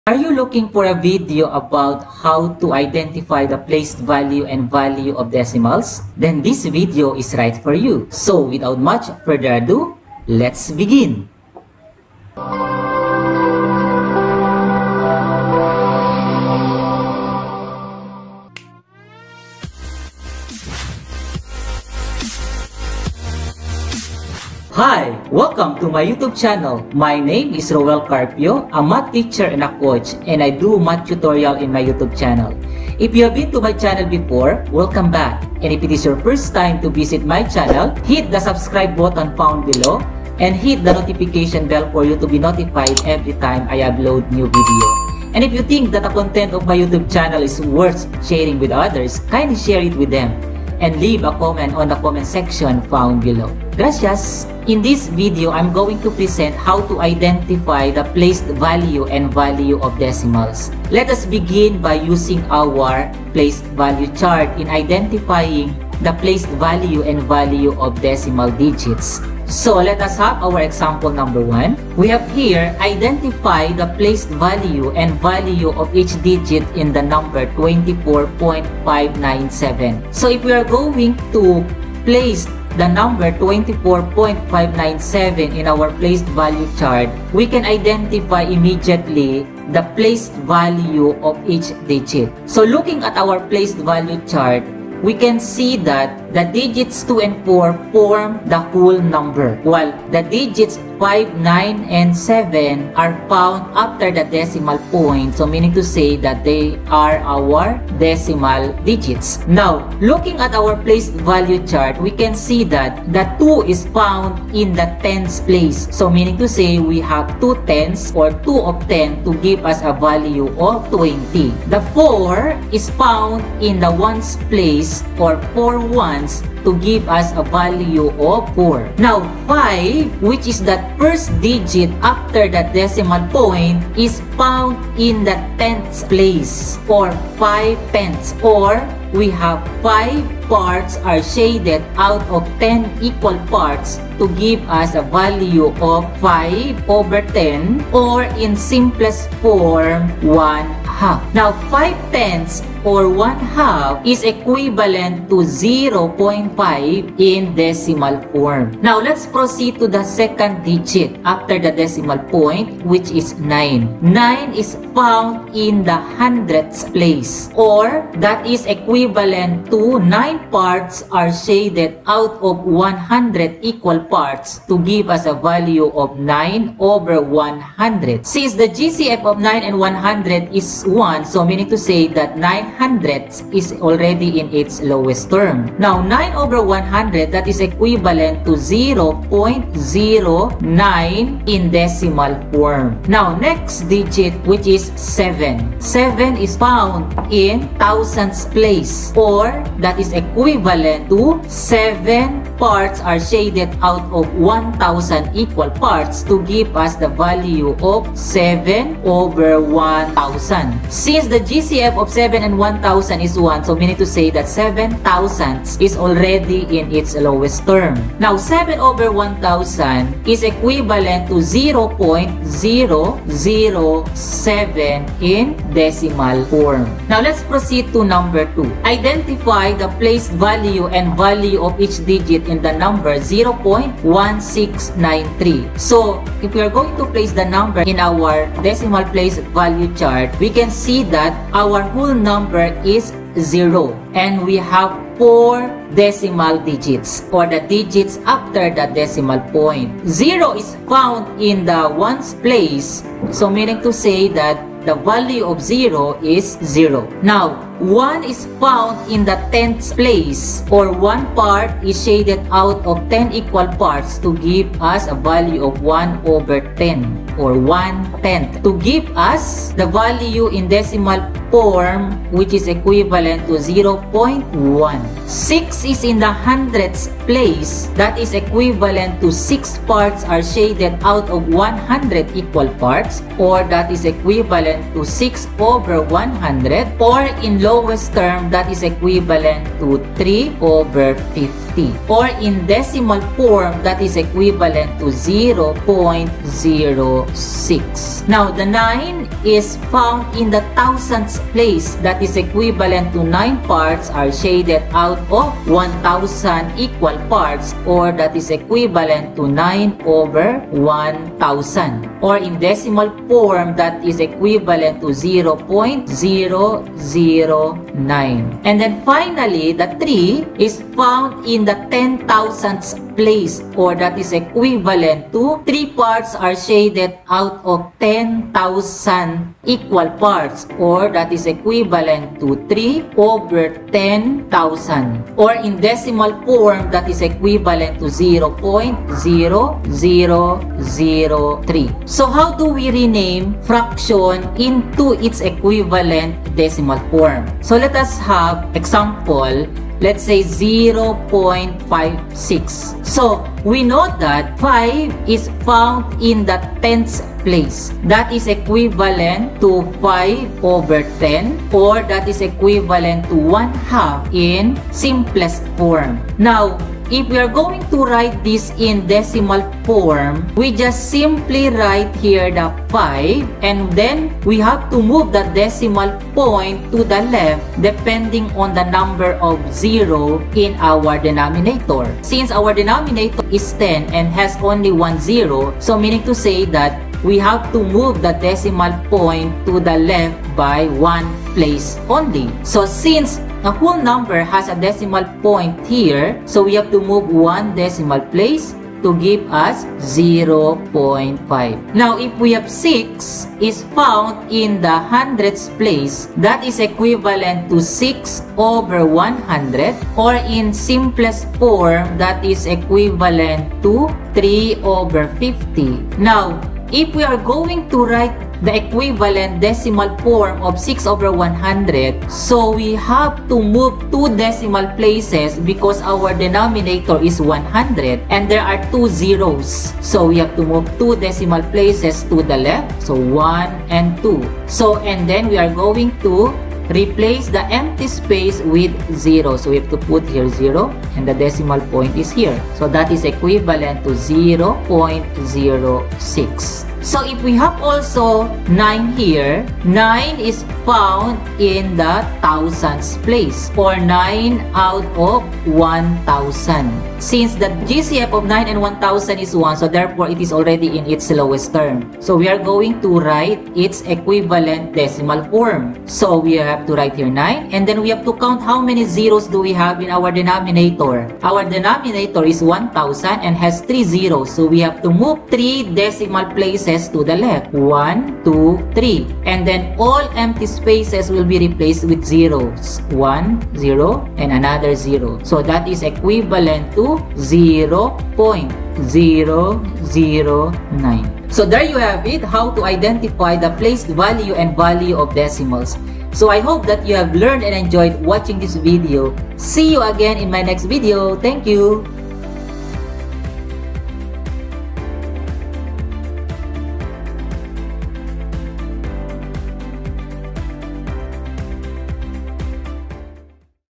HOW TO IDENTIFY THE PLACE VALUE AND VALUE OF DECIMALS ǀ RCMTv
In this video, I am presenting how to identify the place value and value of decimals. Identifying the place value of a digit in a given decimal number correctly can help us identify its value both in fraction form and in decimal form. Through this video, the viewers can understand easily how to identify the place value of any digit in a given decimal number as well as its value both in fraction form and in decimal form. Through the use of decimal place value chart, the identification of the place value of a digit in a given decimal number is made easy.